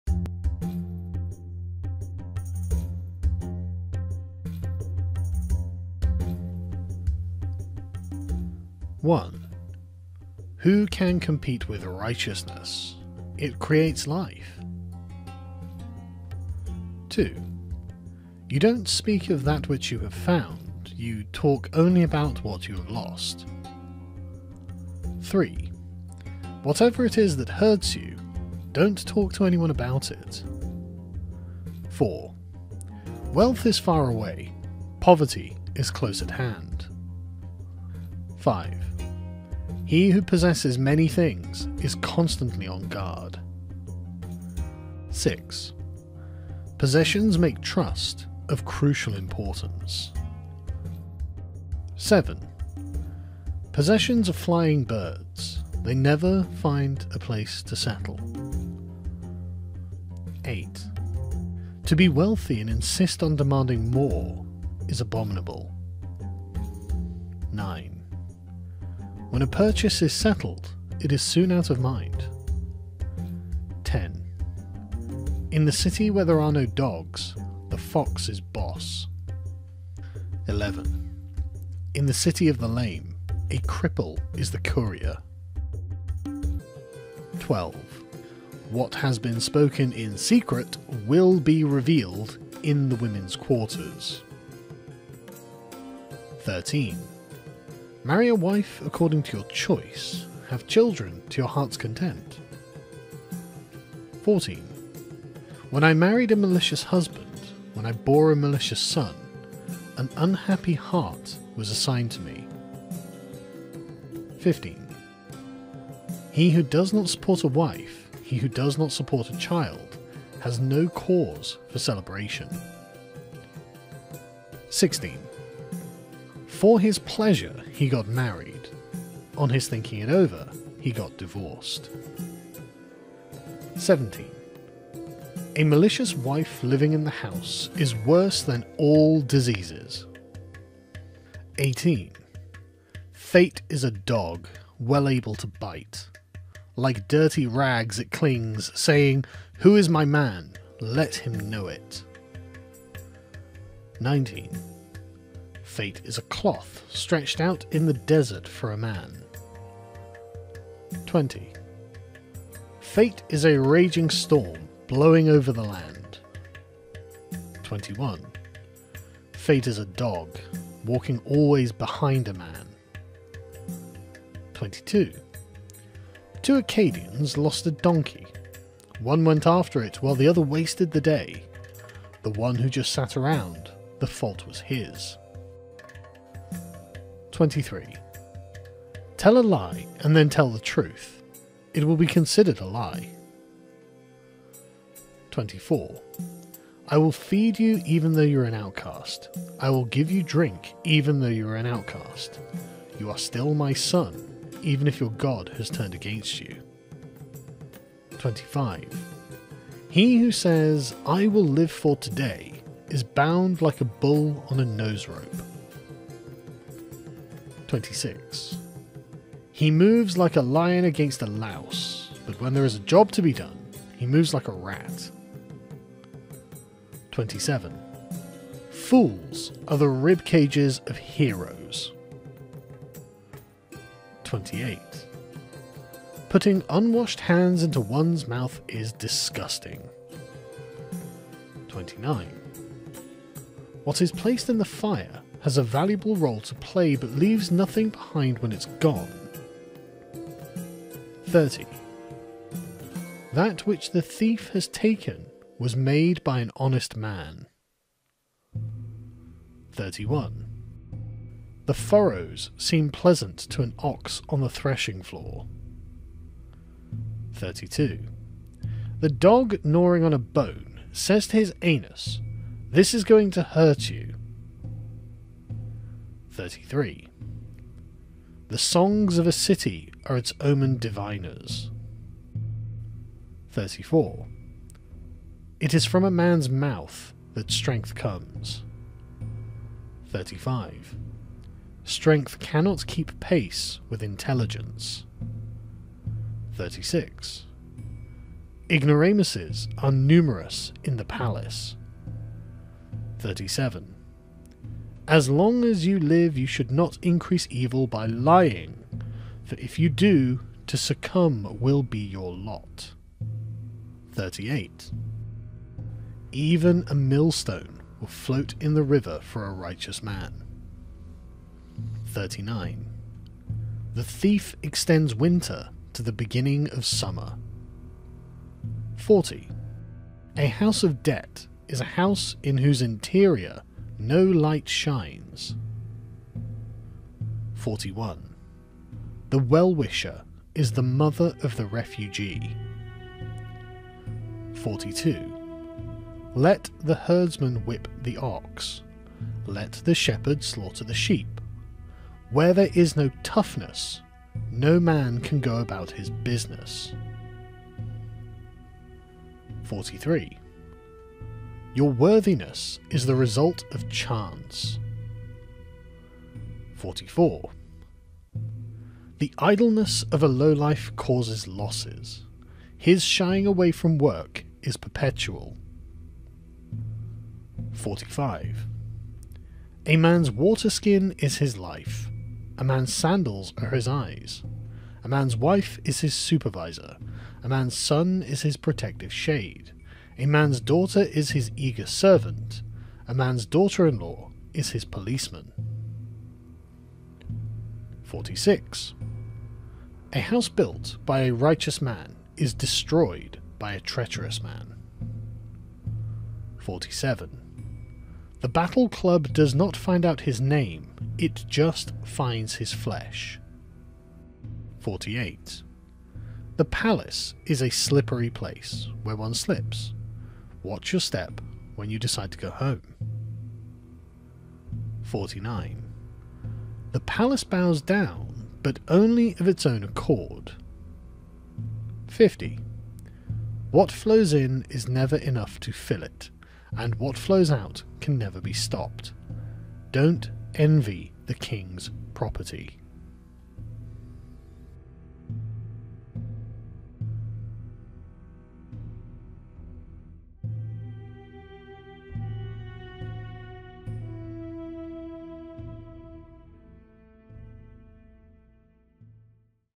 50 Sumerian Proverbs
A collection of Sumerian proverbs that contain wisdom and insight, from around 5,000 years ago.